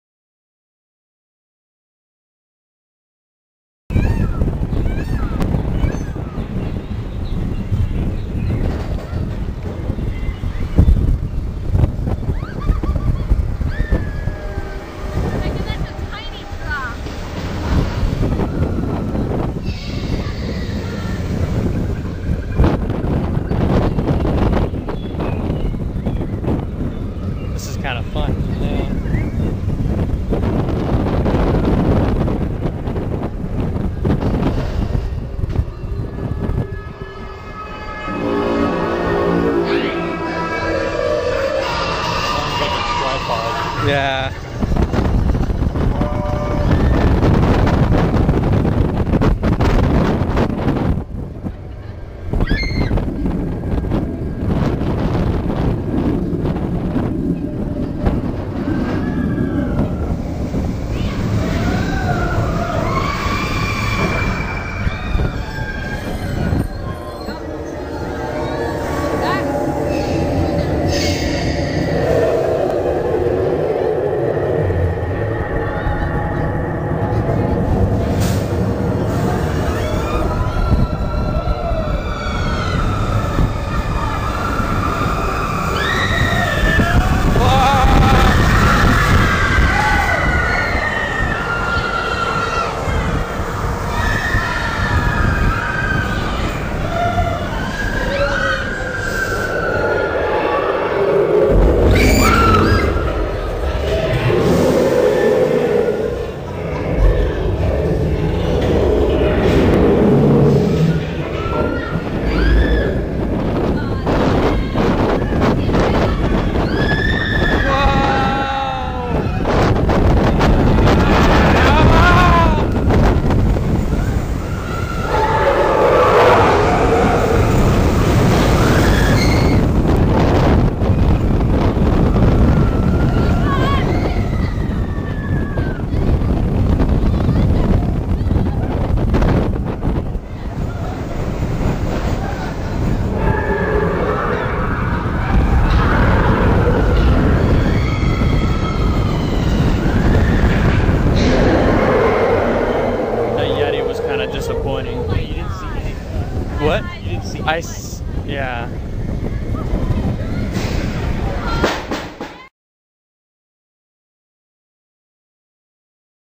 Expedition Everest at Disneyworld
This is an onride video of Expedition Everest located at Disneyworld. This ride proved to be a hit with the Disneyworld audiencem, except for those with epilepsy. Enjoy the ride! for more theme park videos.